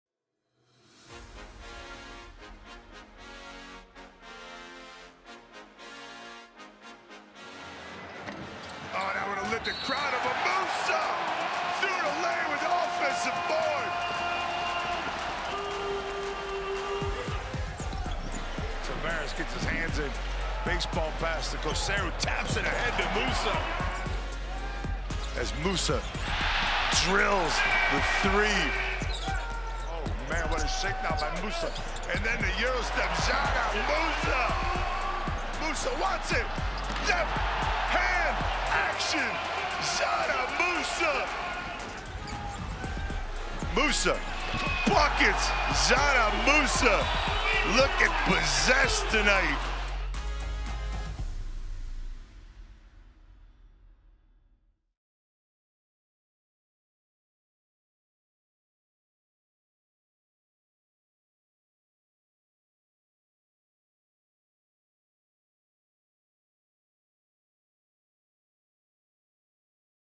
Dzanan Musa | RS Round 19 MVP | 2023-24 Turkish Airlines EuroLeague
A record-breaking performance in a one-of-a-kind game allowed Dzanan Musa of Real Madrid to earn Round 19 MVP honors in the 2023-24 Turkish Airlines EuroLeague Regular Season.